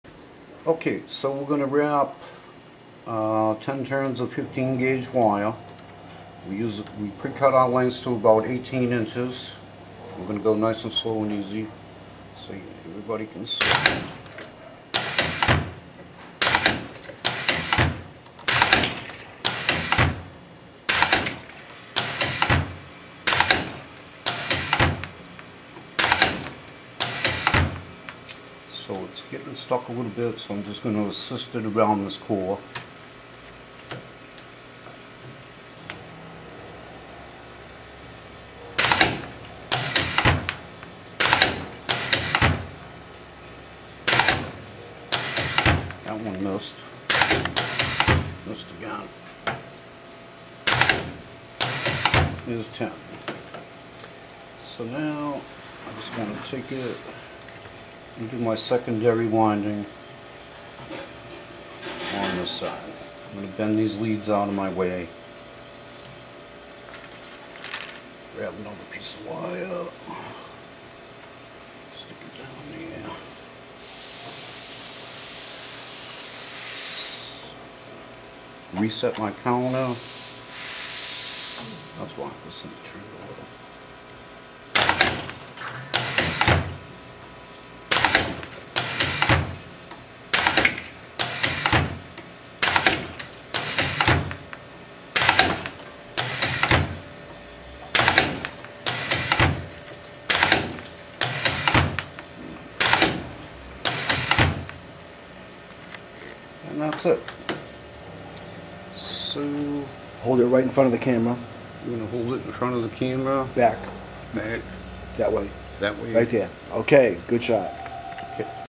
Hustler hook winder made by Gorman Machine Corp
Here is the Hustler Hook winding machine pulling 2 sections of wire at 10 turns of 15 AWG copper wire on each side.